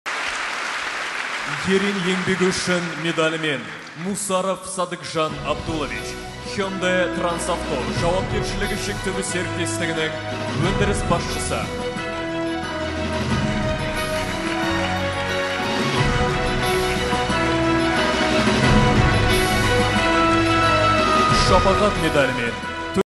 НОВАЯ МЕДАЛЬ В КОМАНДНОМ ЗАЧЕТЕ «АСТАНА МОТОРС» / «АСТАНА МОТОРС» КОМАНДАЛЫҚ ЕСЕБІНДЕГІ ЖАҢА МЕДАЛЬ
16 ноября 2022 года глава государства Касым-Жомарт Токаев подписал Указ о награждении государственными наградами Республики Казахстан. За значительные достижения и особые заслуги перед Республикой Казахстан к награде «Ерен еңбегі үшін» представлен Мусаров Садыкжан Абдулович, руководитель производства Hyundai Trans Auto!

Садыкжан Абдулович работает в структуре «Астана Моторс» почти 20 лет! За это время он прошел карьерный путь от автомеханика SUBARU MOTORS Almaty до руководителя производства Hyundai Trans Auto.

Коллеги знают Садыкжана Мусарова как отзывчивого, грамотного, ответственного специалиста. Именно благодаря своим профессиональным качествам и навыкам в прошлом году Садыкжан Абдулович стал руководителем производства Hyundai Trans Auto. В том же 2021 году заслуги Садыкжана Абдуловича перед отраслью отметили почетным знаком «Еңбегім еліме»!

Коллектив «Астана Моторс» поздравляет Садыкжана Мусарова с присвоением государственной награды и желает дальнейших профессиональных успехов на благо компании и страны!
2022 жылғы 16 қарашада мемлекет басшысы Қасым-Жомарт Тоқаев Қазақстан Республикасының мемлекеттік марапаттарымен марапаттау туралы жарлыққа қол қойды. Қазақстан Республикасы алдындағы елеулі жетістіктері мен сіңірген айрықша еңбегі үшін Hyundai Trans Auto өндірісінің жетекшісі Мусаров Садықжан Абдулович «Ерен еңбегі үшін» марапатына ұсынылды!

Садықжан Абдулович «Астана Моторс» құрылымында 20 жылға жуық қызмет етіп келеді! Осы уақыт ішінде ол SUBARU MOTORS Almaty автомеханигінен Hyundai Trans Auto өндірісінің жетекшісіне дейінгі мансаптық жолмен өтті.

Әріптестері Садықжан Мусаровты қайырымды, білгір, жауапты маман ретінде біледі. Өзінің кәсіби қасиеттері мен дағдыларының арқасында былтыр Садықжан Абдулович Hyundai Trans Auto өндірісінің жетекшісі атанды. Тап сол 2021 жылы Садықжан Абдуловичтің салаға сіңірген еңбегі «Еңбегім еліме» құрмет белгісімен атап өтілді!

«Астана Моторс» ұжымы Садықжан Абдуловичті мемлекеттік марапаттың берілуімен құттықтайды және компания мен ел игілігі жолында одан әрі кәсіби жетістіктерге жетуді тілейді!